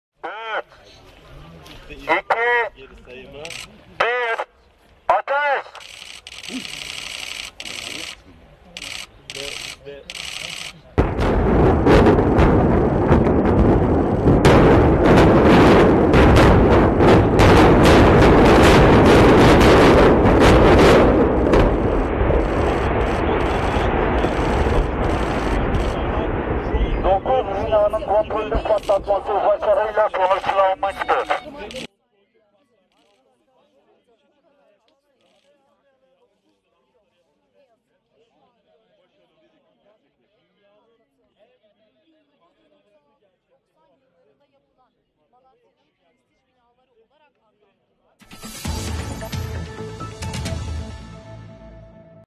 В Турции взорвано девять жилых домов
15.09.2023. В провинции Малатья, Турция, точечно взорвали девять 15-этажных домов, которые стали аварийными для проживания после серии землетрясений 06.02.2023 года. Тогда из-за землетрясений в провинции Малатья серьёзно пострадали 36.000 домов, на сегодня 6.000 домов уже снесли... Сносимые дома построены в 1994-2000 годы... Турчанам за счёт государства и строительных компаний предоставляются новые дома...
Так бы в России сносили аварийное жильё и отстраивали новое, раздавая бесплатно россиянам, но куда там... Россияне продолжают жить в сгнивших серо-плесневелых дощатых бараках сталинской постройки 1929-30 годов, не говоря про хрущёвки и аварийные панельки... Кому он нужен этот россиянин?